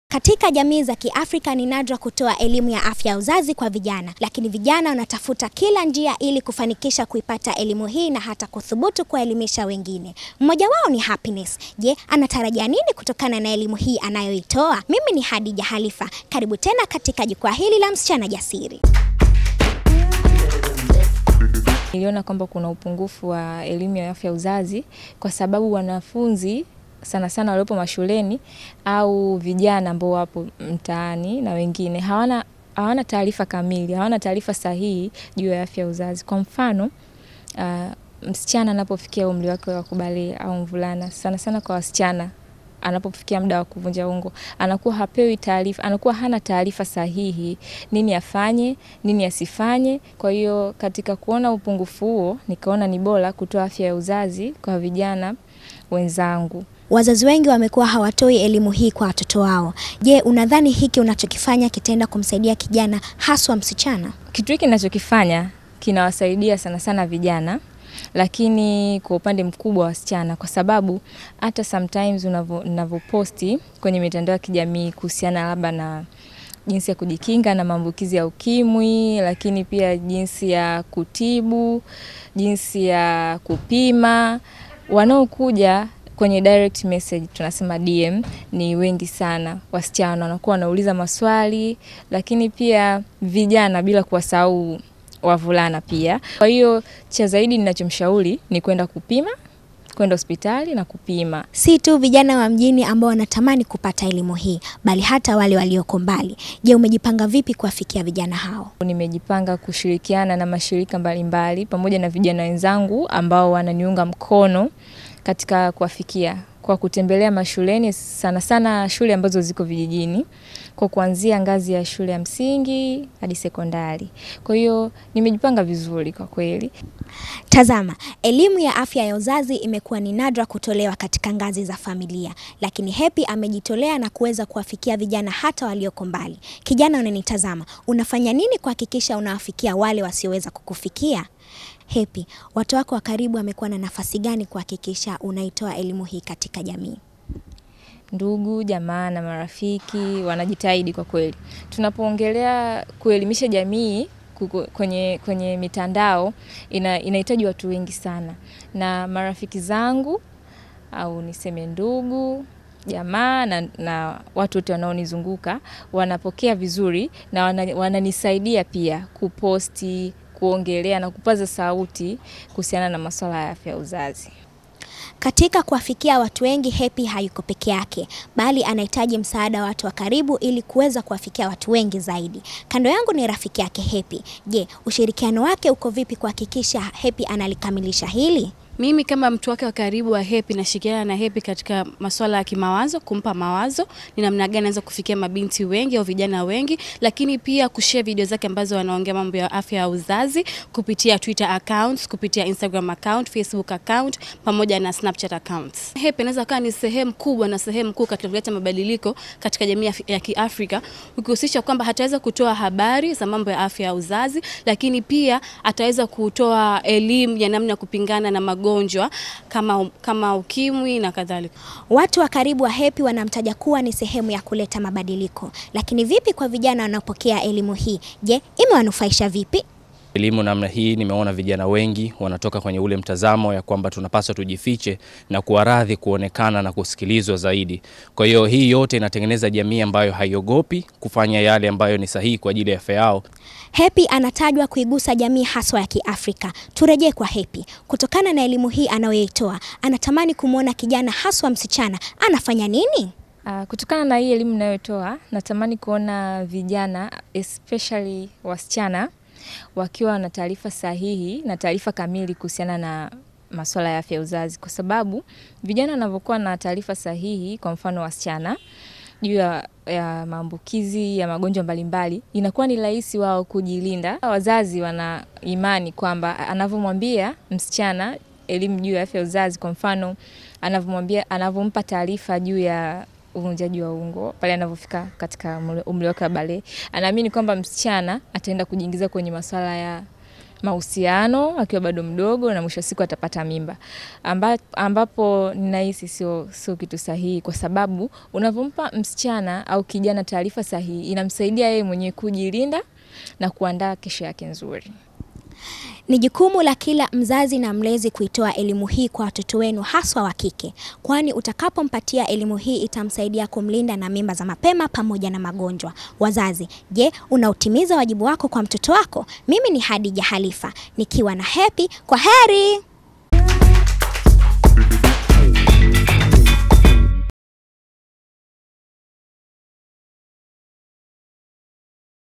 Happy Malunde - Mwanaharakati anayepambania afya ya uzazi ya vijana wenzake
Kukua kwa teknolojia kunawasadia vijana kupashana habari na hata kuelimishana kwa vitu mbalimbali hivi ndivyo Happy Malunde anavyoitumia teknolojia katika kuwaelimisha vijana juu ya elimu ya afya ya uzazi huku vijana wengine wakipata nafasi ya kuzielezea changamoto zao.

Mtangazaji Chipukizi Hadija Halifa amekutana na Happy na kuzungumza naye.